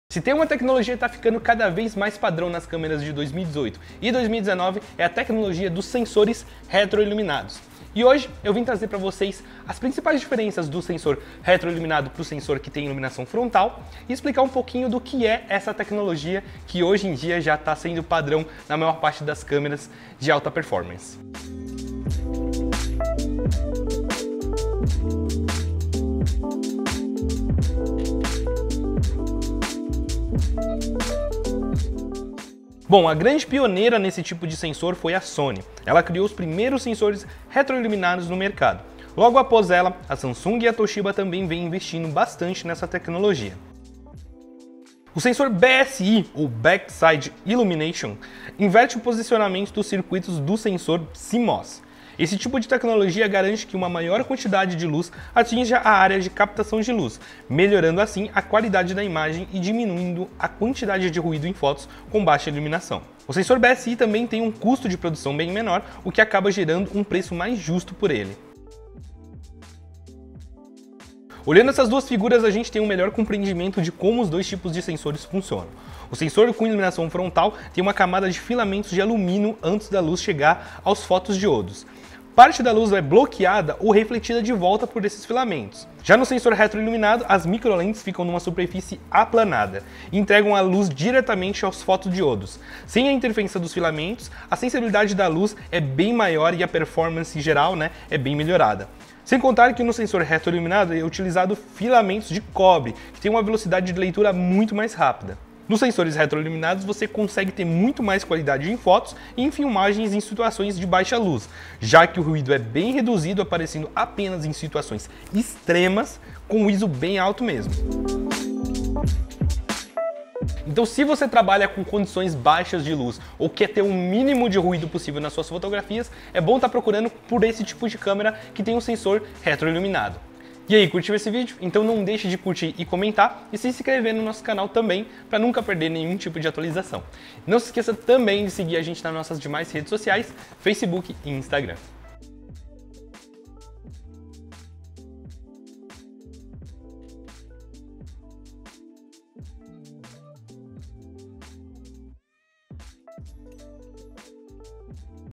Conheça os Sensores Retroiluminados
Bora conhecer as diferenças dos novos sensores cmos retroiluminados pro cmos convencional? Nesse vídeo trago pra vocês as diferenças mais contrastantes entre eles, demonstrando em câmeras de qualidade equivalente a diferença em relação ao ruído em cada um dos sensores.